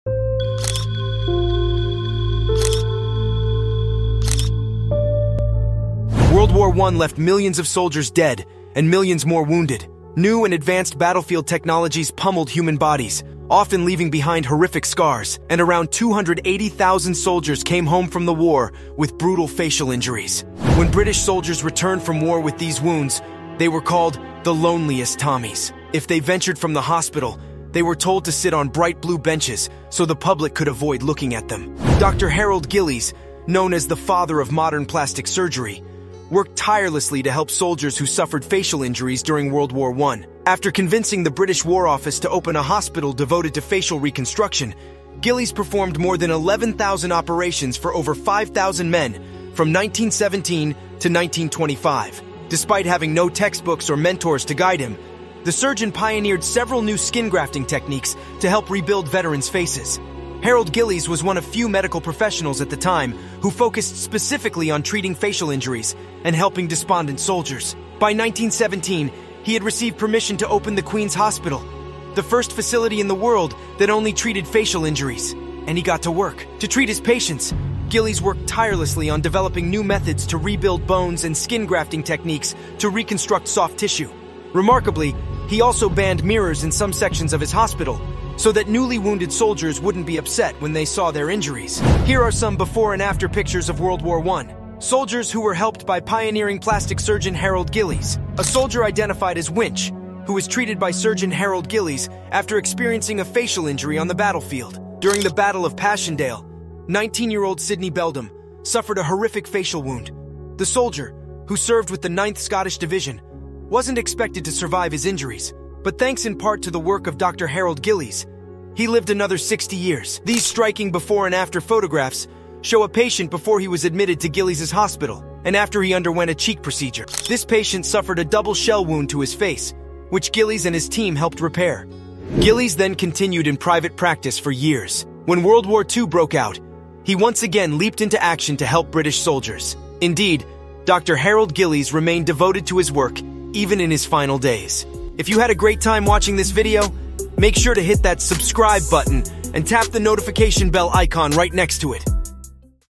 World War I Soldiers returned with brutal facial injuries
World War I left millions of soldiers dead and millions more wounded. New and advanced battlefield technologies pummeled human bodies, often leaving behind horrific scars. And around 280,000 soldiers came home from the war with brutal facial injuries.

Dr. Harold Gillies Known as the "father of modern plastic surgery," worked tirelessly to help soldiers who suffered facial injuries during World War I.